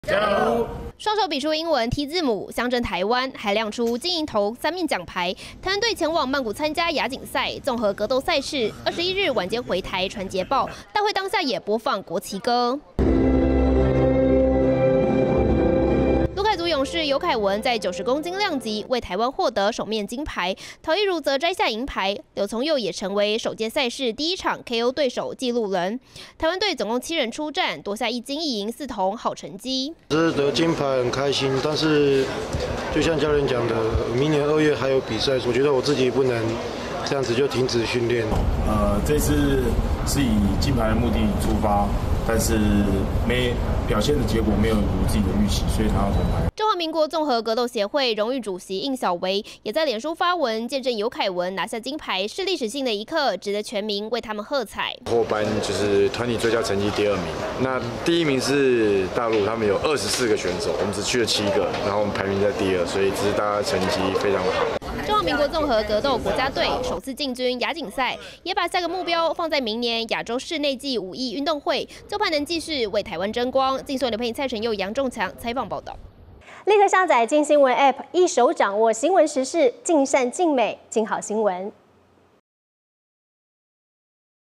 為台爭光！綜合格鬥亞錦賽尤凱文替台摘首面金牌｜鏡新聞
台灣隊在亞錦賽綜合格鬥賽事傳捷報，由魯凱族勇士「尤凱文」為台灣奪下首面金牌，而陶亦儒則摘下銀牌。台灣隊總共七個人前往曼谷參加亞錦賽，奪下一金、一銀與四銅的好成績，昨日（8/21）晚間回到台灣，選手也發表各自感言，未來將把目標放在明年二月的賽事。